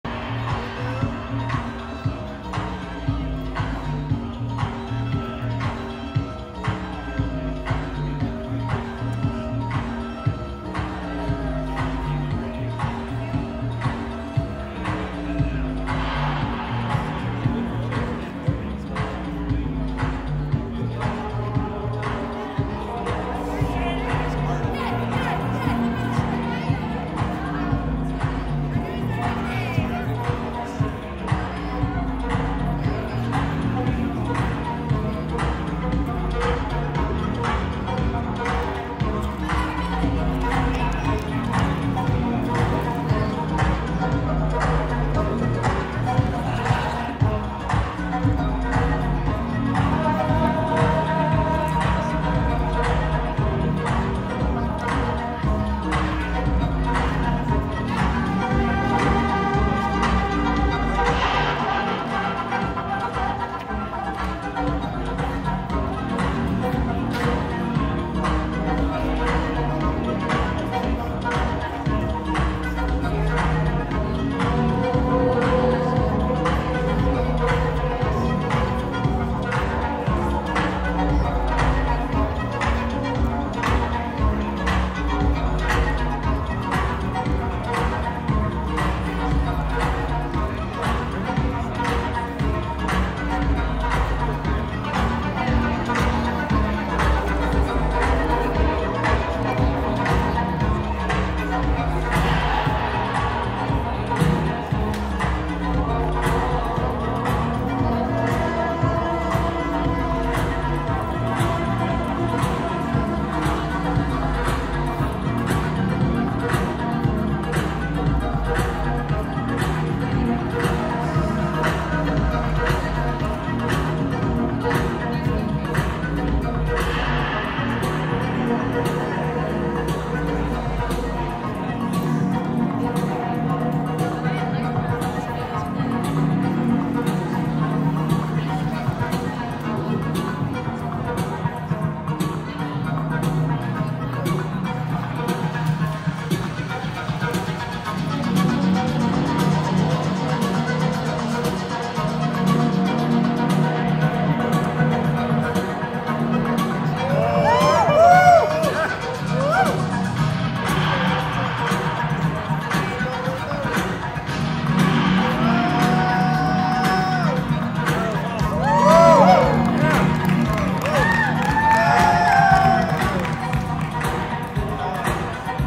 A7SIII Fire dancing ft. Thomas (solo) @ The Concourse
Fire dancing ft. Thomas (solo) @ The Concourse in Knoxville, Tn

Shot on Sony a7siii